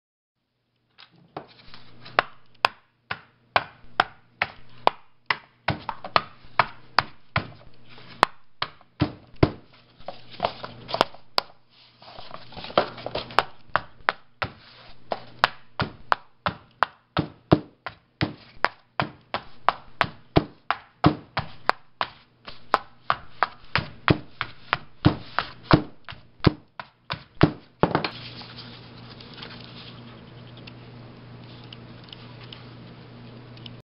HOW TO FLATTEN AN EGG CARTON
SAVE SPACE, MAKE AN EGG CARTON FLAT WITH A HAMMER.